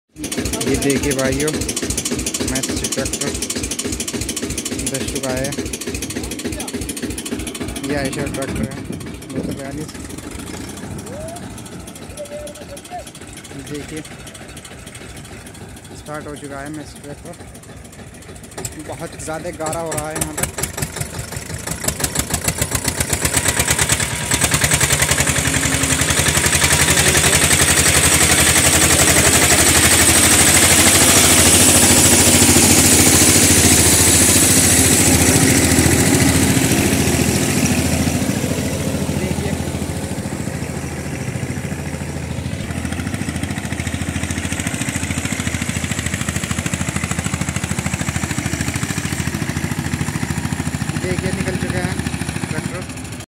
Massey 35 को Eicher 242ने धसन में से खींचा💪🔥
my videos